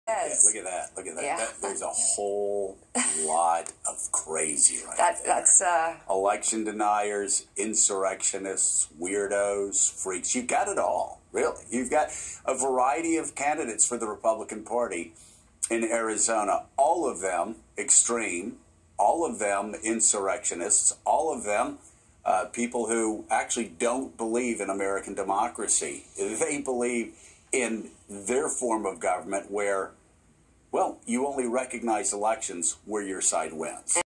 AZ republicans elect election deniers, insurrectionists, weirdos, and freaks. Do better in November
Vote blue to save American democracy, no lie. Vote for people who believe in government and want to represent you in Congress, not people who lie to you to enrich themselves.  8/5/22